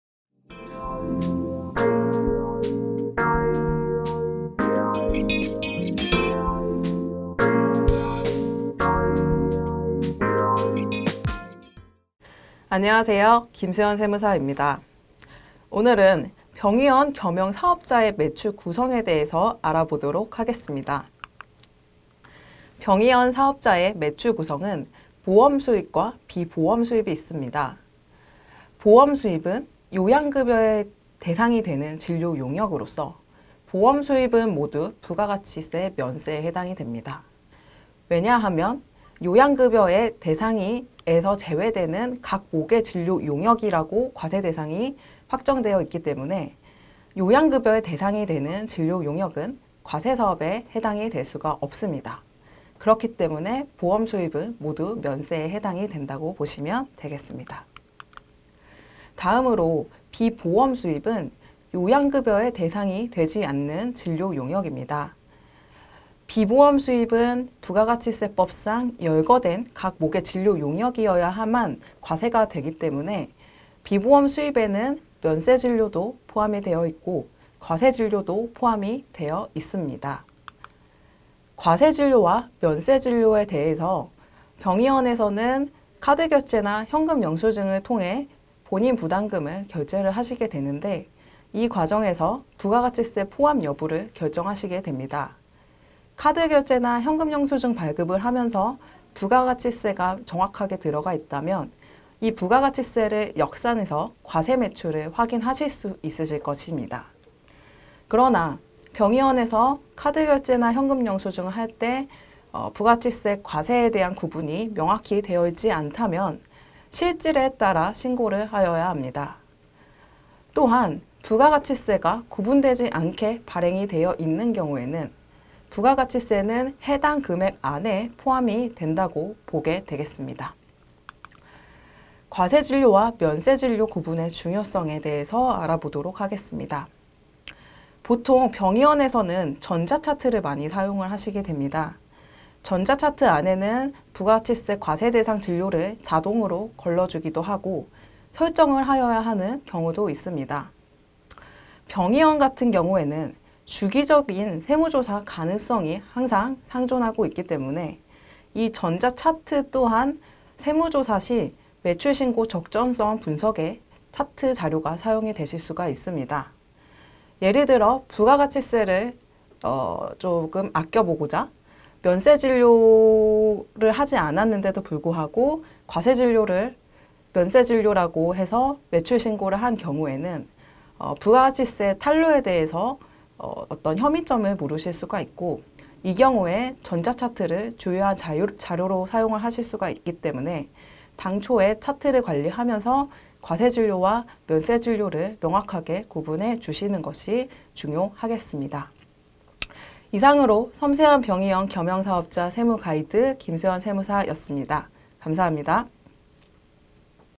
#5 병의원 겸영사업자의 매출 구성 (김수현세무사)
5. 섬세한 병의원 겸영사업자(성형외과, 피부과, 치과) 세무가이드
- 병의원 겸영사업자의 매출 구성
      1) 보험수입과 비보험 수입
      2) 비보험수입 중 면세진료와 과세진료
      3) 과세진료용역 확인을 위한 전자차트 관리법